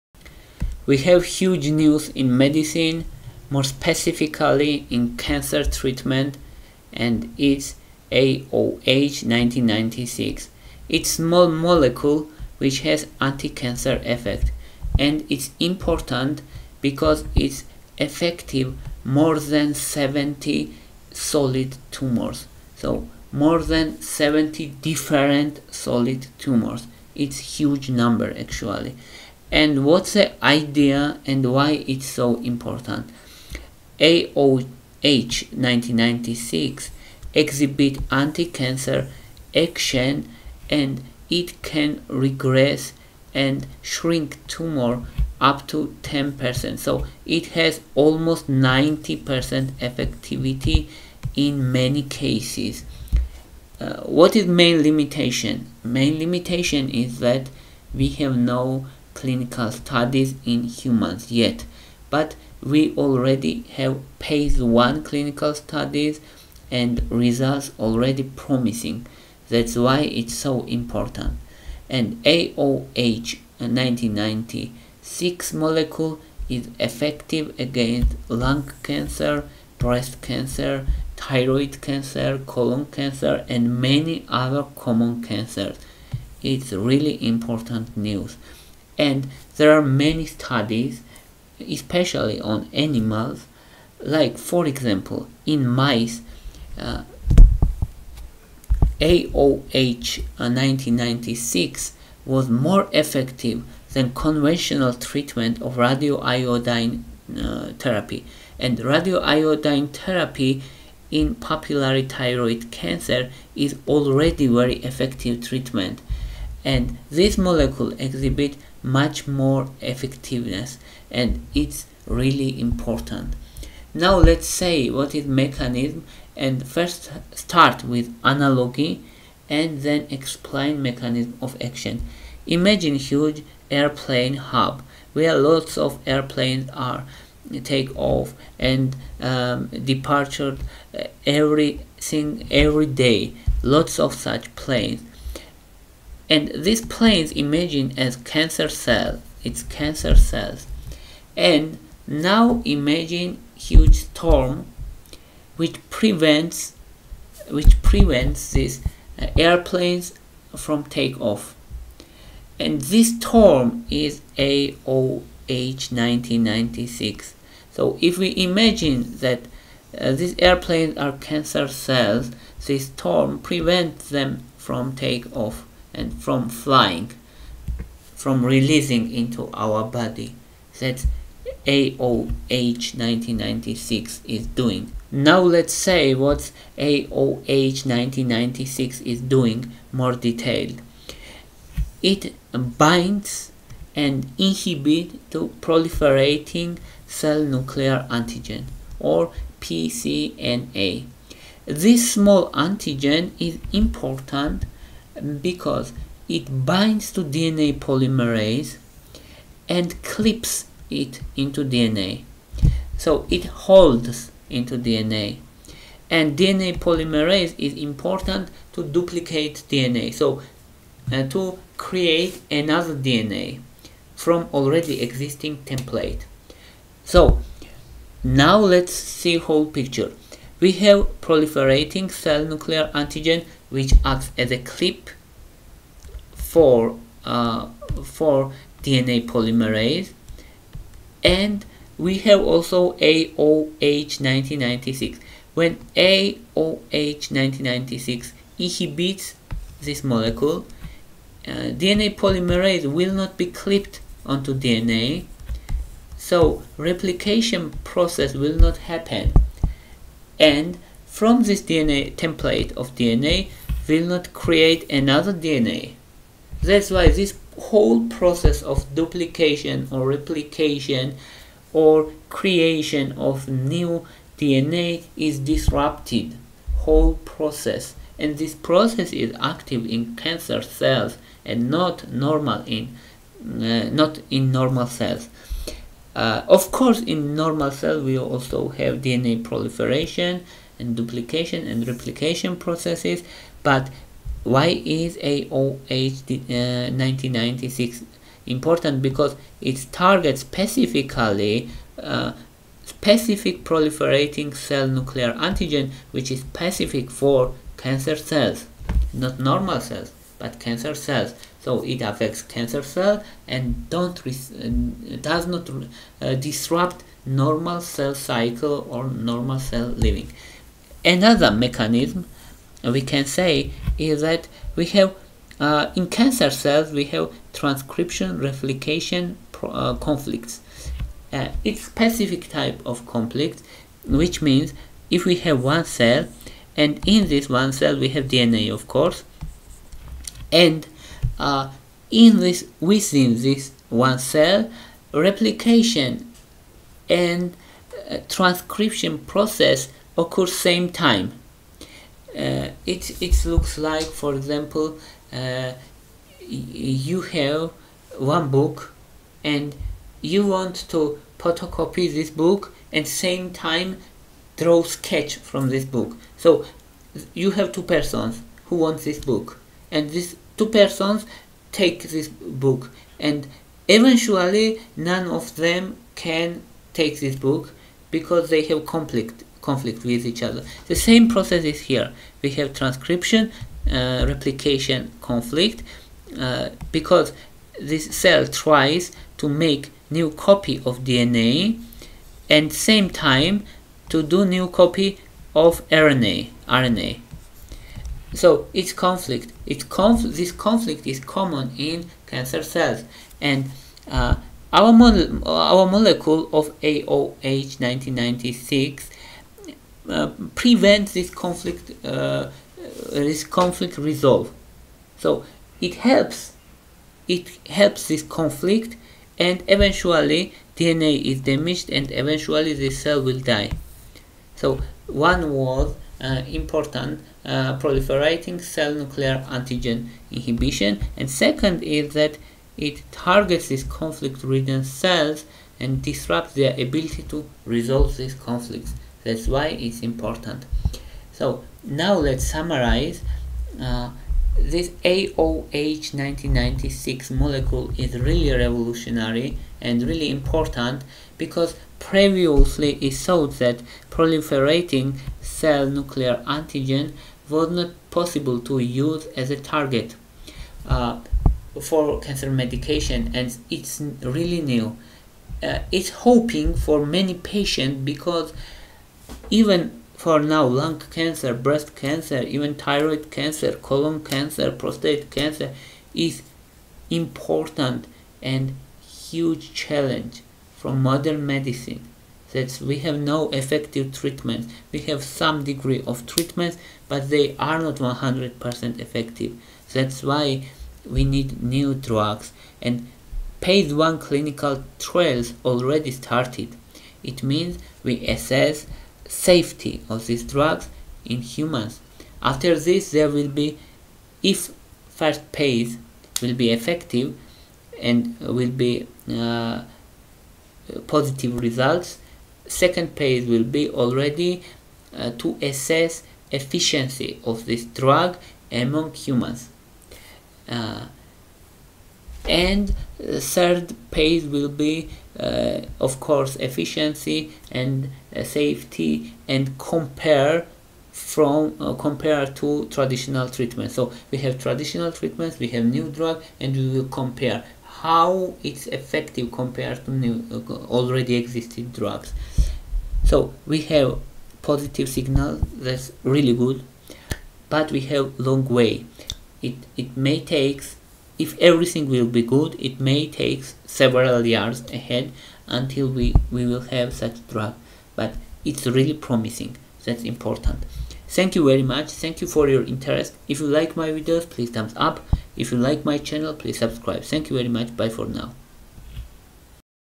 AOH1996 - Anti-cancer Molecule. Groundbreaking news in Cancer treatment
What is AOH1996 and why is it a promising new drug in cancer treatment?
How does AOH1996 work and what is its mechanism of action in treating cancer?
How does AOH1996 target the proliferating cell nuclear antigen (PCNA)?
What are transcription-replication conflicts and how does AOH1996 affect these conflicts?
How does AOH1996 affect the process of DNA replication in cancer cells?
What are the potential impacts of AOH1996 on different types of solid tumors?
What is the current status of clinical trials involving AOH1996?
Highlights:

AOH1996 is a novel targeted chemotherapy that targets a cancerous variant of PCNA and has shown promise in annihilating all solid tumors in preclinical research.
The mechanism of action of AOH1996 involves disrupting DNA replication in cancer cells by targeting the cancerous form of PCNA.
AOH1996 also has a role in transcription-replication conflicts, where it targets cancer cells in which transcription and replication occur simultaneously, disrupting conflict resolution and inhibiting DNA polymerase.
In animal models, AOH1996 has shown to be more effective in treating thyroid papillary tumors than standard radioactive iodine treatment.
In vitro studies have shown AOH1996 can kill up to 90% of human lung cancer cells.
AOH1996 is currently in Phase 1 clinical trials assessing its safety in healthy individuals.